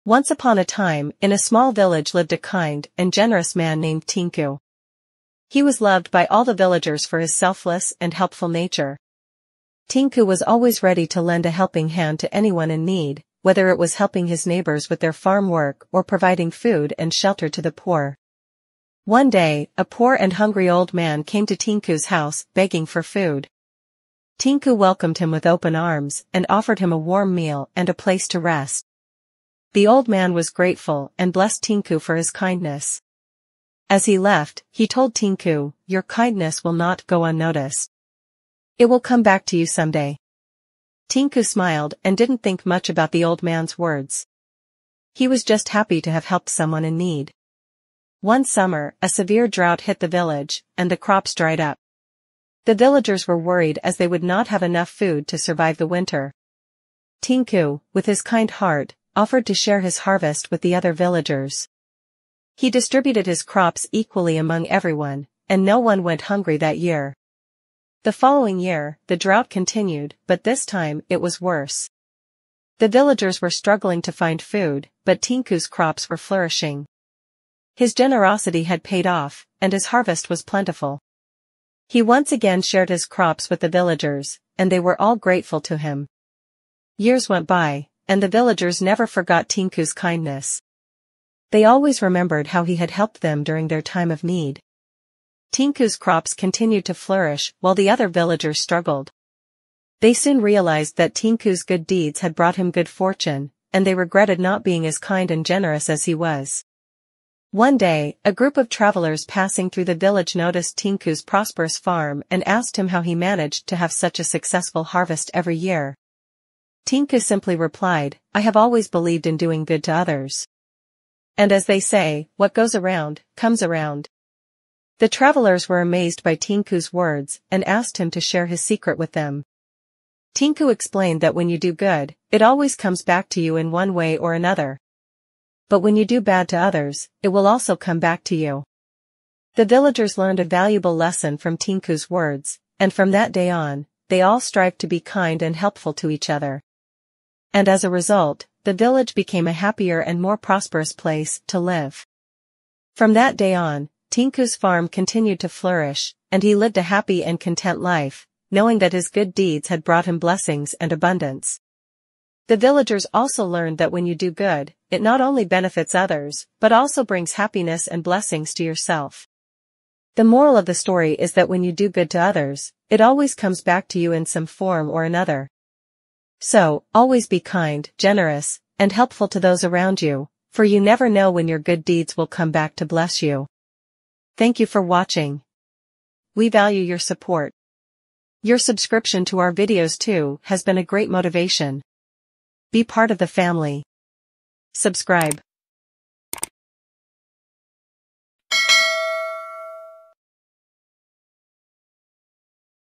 Tinku story with a moral lesson for kids and teenagers | Story time |Bedtime stories | Fairy tales
The story is about Tinku who was kind hearted with a moral lesson that when you do good to others, it always comes back to you in some form or another. The video encourages us to always be kind, generous, and helpful to those around us.